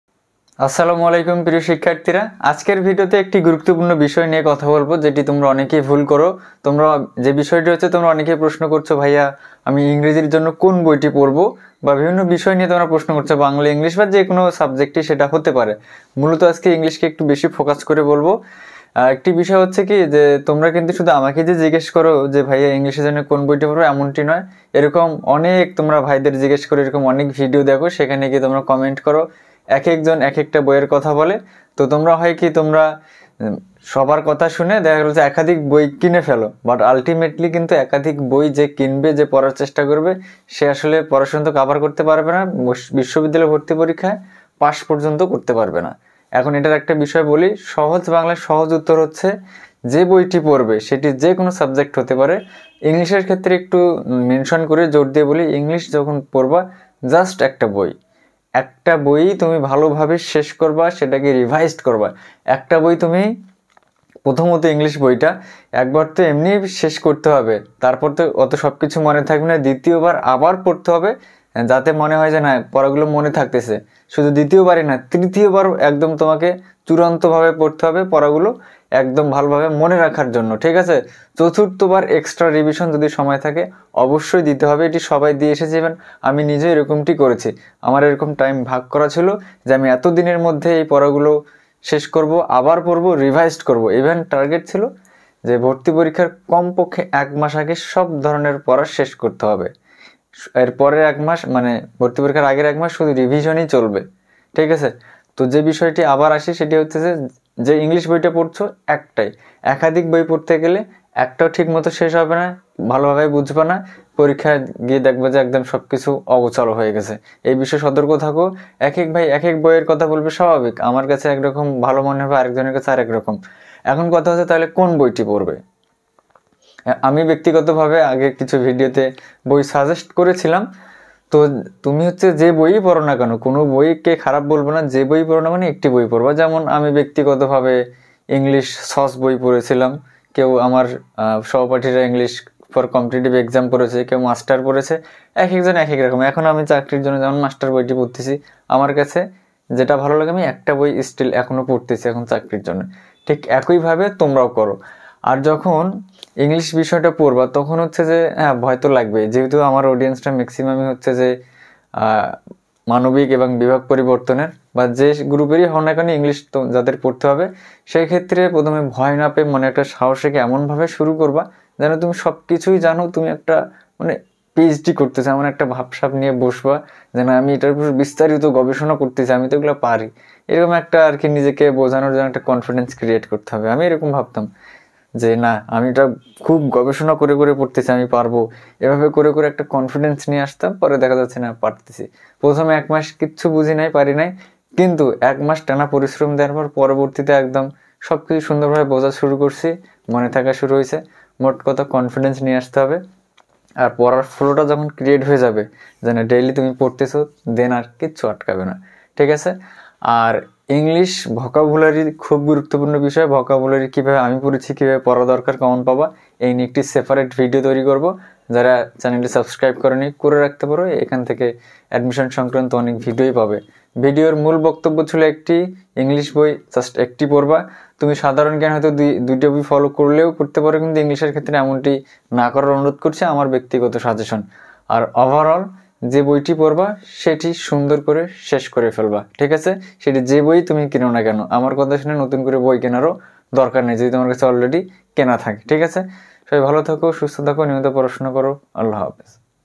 বিশ্ববিদ্যালয় ভর্তি প্রস্তুতিতে একাধিক বই ফলো করবে না।University Admission Test.
আজকের ভিডিওতে মূলত আলোচনা করা হয়েছে বিশ্ববিদ্যালয় ভর্তি প্রস্তুতি সংক্রান্ত বিষয় নিয়ে।